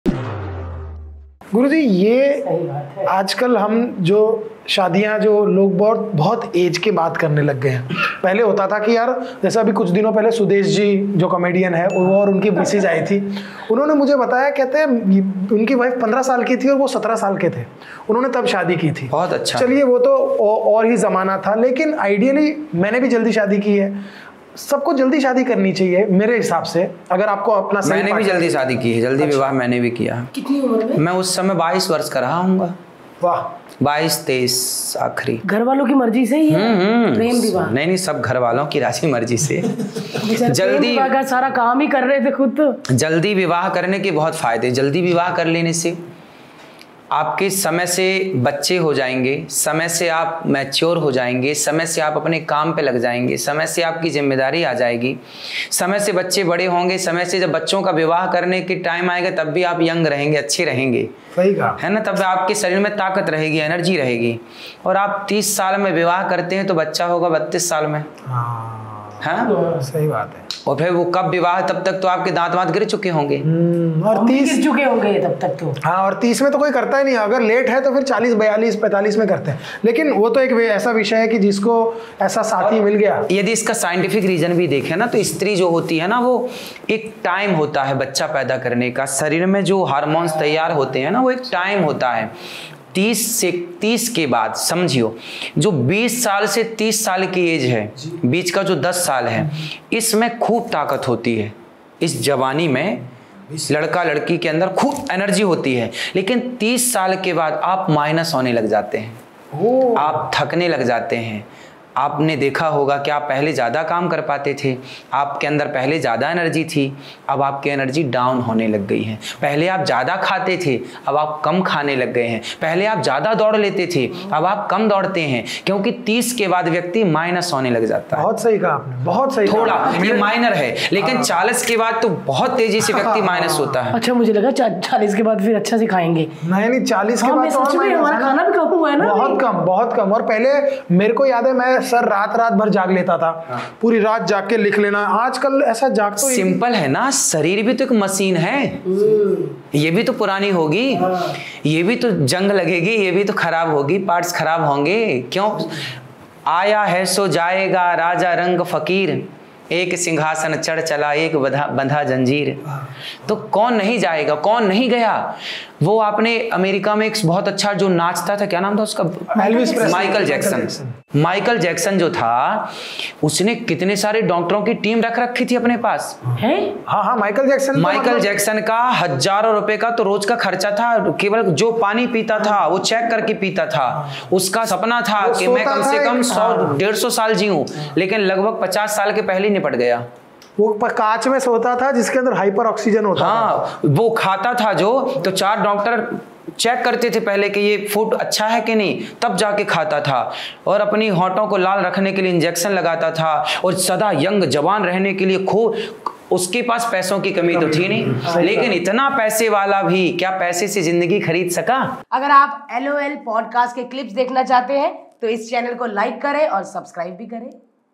Why You Should Marry Early | Aniruddhacharyaji Maharaj's Advice
In this video, Aniruddh Acharya Ji emphasizes the importance of marrying at a significant age. He explains how early marriage helps in developing a better understanding of relationships, responsibilities, maturity, and parenthood. Acharya Ji also explores the science behind why women should conceive at a certain age. Drawing from the example of Michael Jackson, he illustrates how, despite having the best of everything—world-class professionals, resources, and fame—Jackson didn’t experience longevity in life. This serves as a powerful reminder of how certain life choices can impact our future.

Watch Bharti TV Clips for more enlightening content like this!